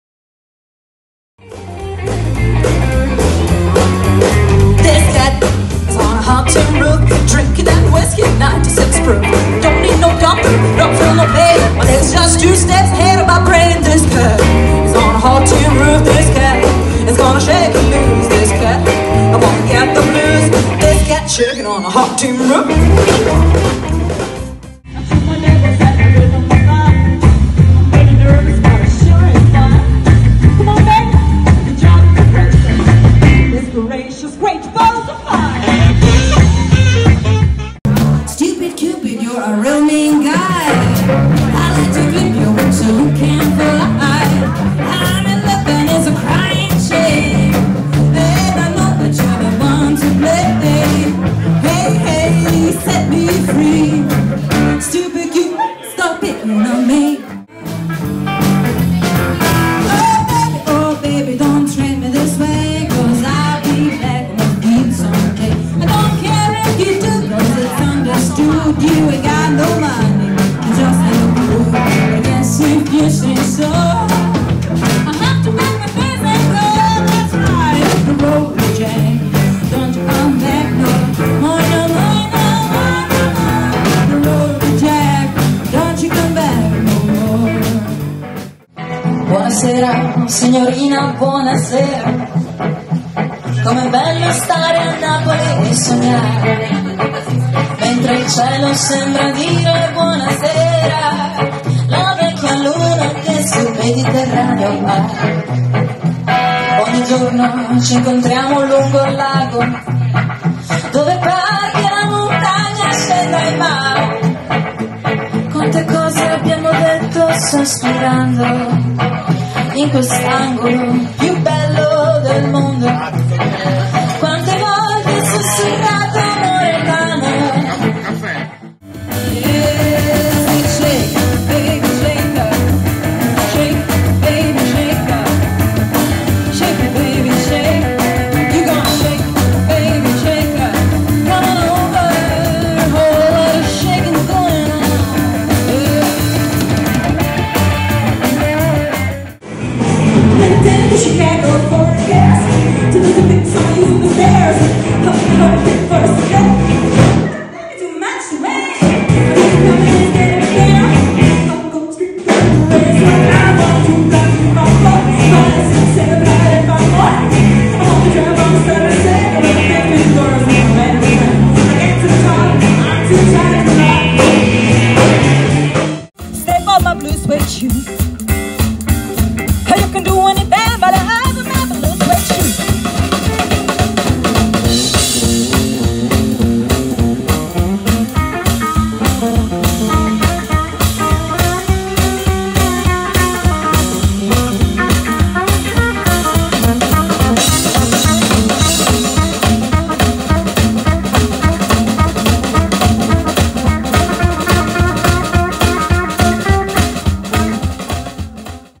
Florie & the Lazy Cats Showreel, 2024
Live band con repertorio swing, rock'n'roll e rockabilly anni 50-60, inglese e italiano
Fiorenza Rosanna: voce
Leonardo Ceralli: chitarra
Maurizio Torchio: basso
Giulio Ferro: batteria